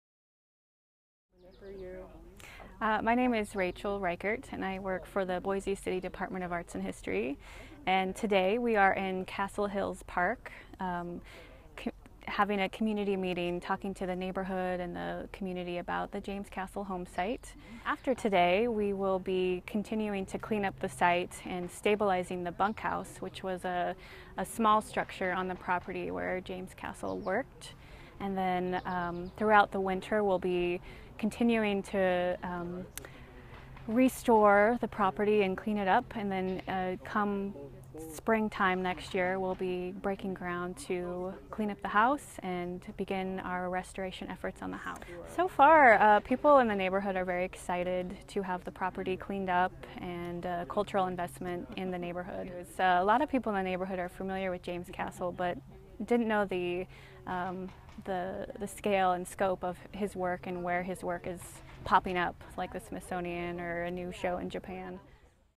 Neighborhood meeting for the James Castle homesite
The City of Boise recently purchased the former home of celebrated self-taught artist James Castle. The staff of the city's Department of Arts and History hosted a neighborhood meeting on July 29 to tell neighbors more about the project and about Castle's significance.